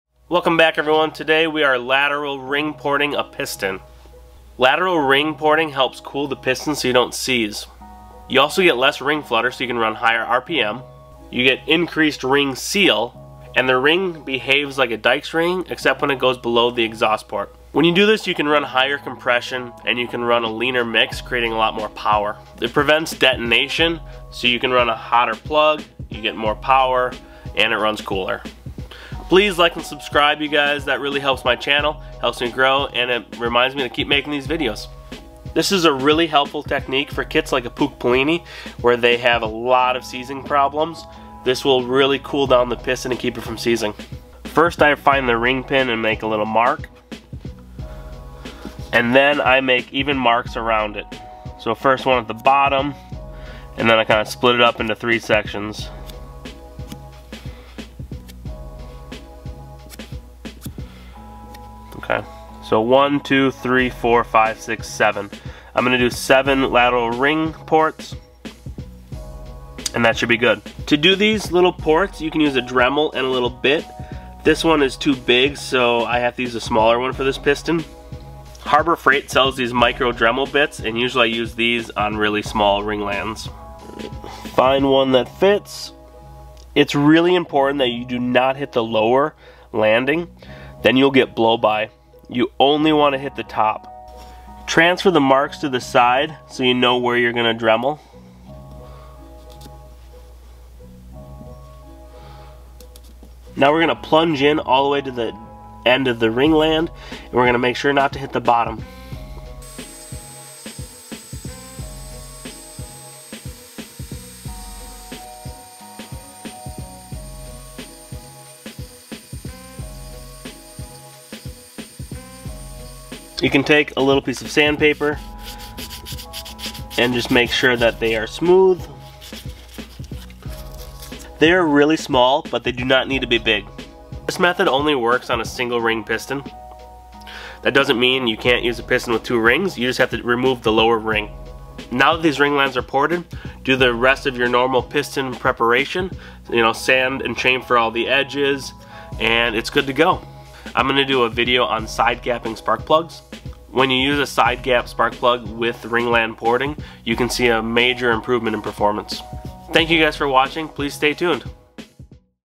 Lateral Gas Porting on a 2 stroke to gain hp
Lateral gas porting was taught to me by Matt Kneen. He built winning snowmobiles for years and used this technique in them. If you want to read a thread he is in this is a great one.

I do this to all of my personal builds and see a great improvement with increased compression, lower temps, and larger carbs.

If you want some micro bits, I use these are they are good. but expect to break a couple because they are so small.